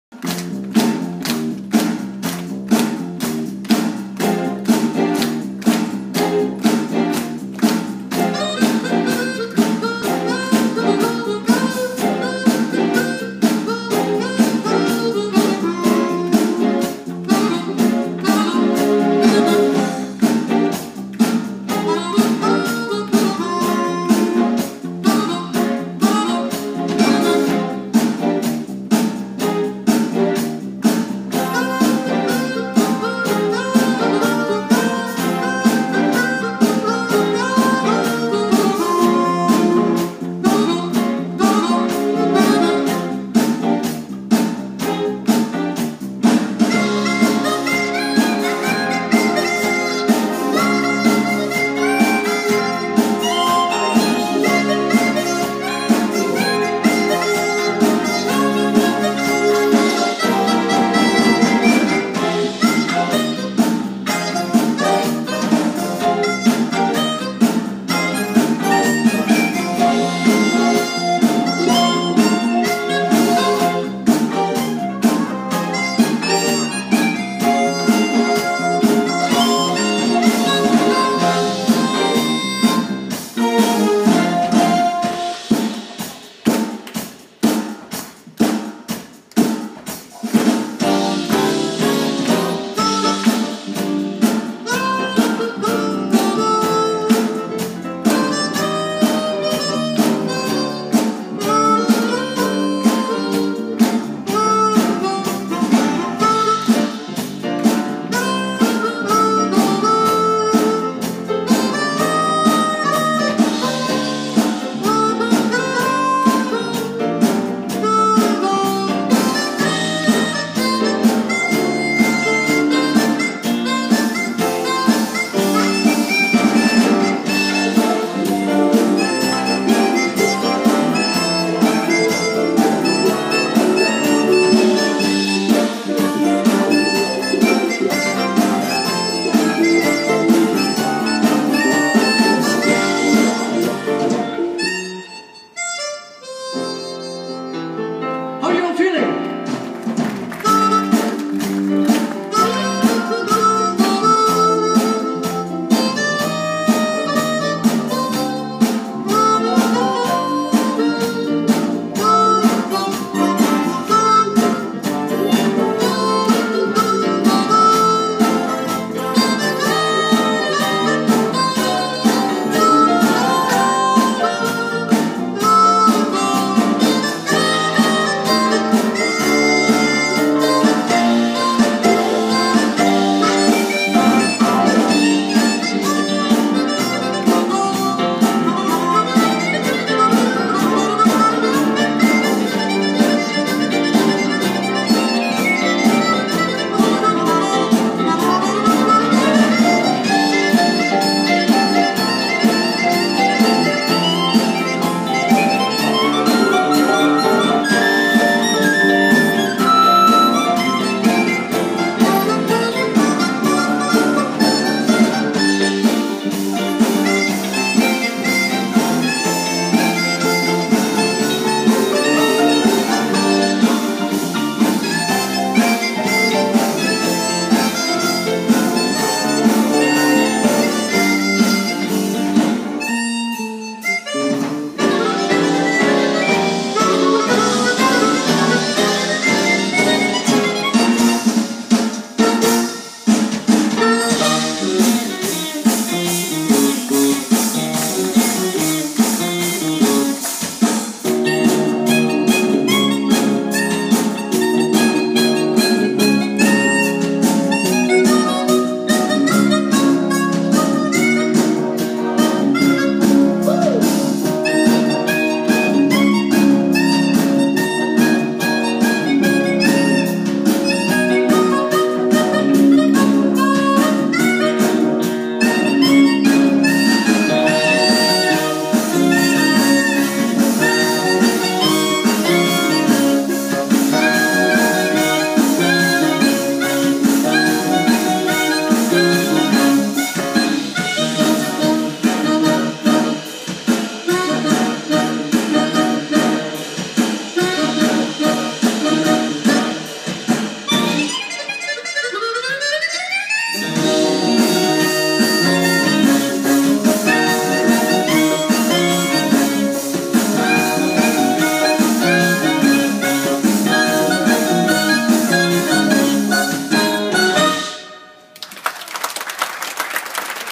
Michael Jackson medley
Performed by Cy Leo & ms. Volendam show band